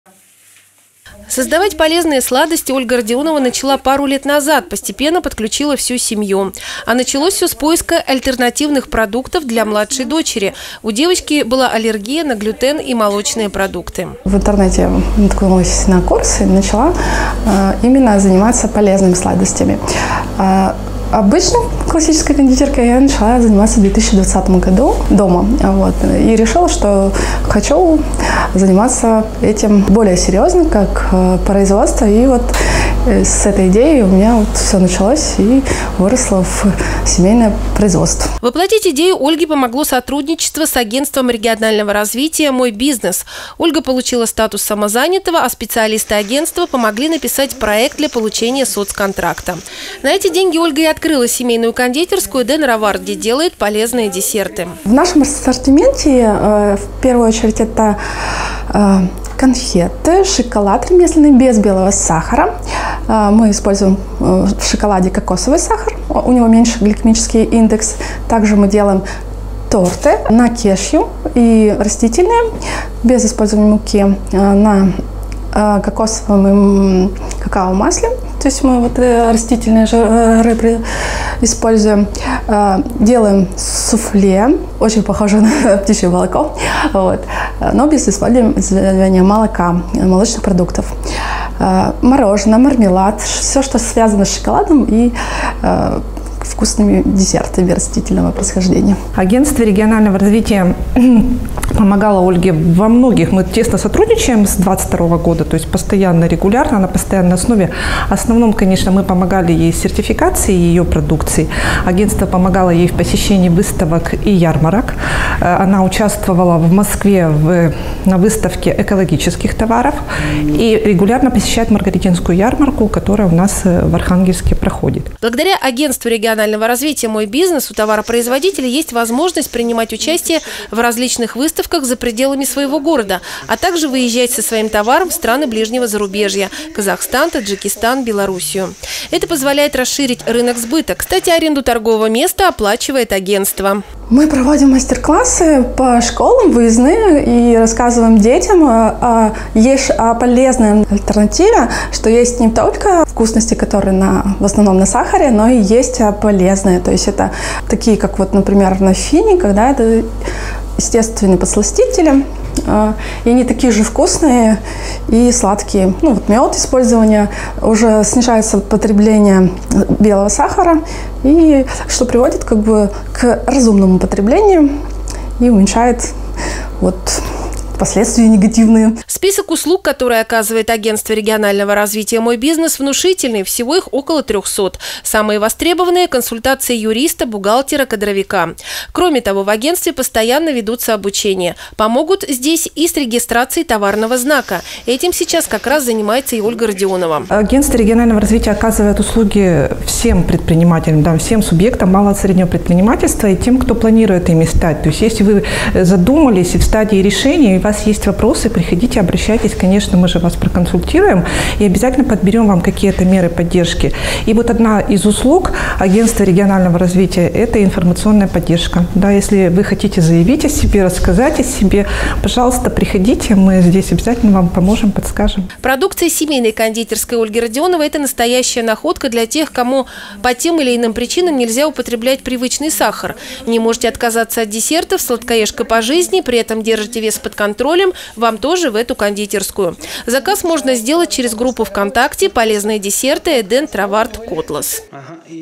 Полезные сладости
Полезные сладости бывают. Именно такие делает в своей семейной кондитерской  котлашанка Ольга Родионова. Её десерты не содержат белый сахар. А воплотить мечту о собственном производстве помог социальный контракт.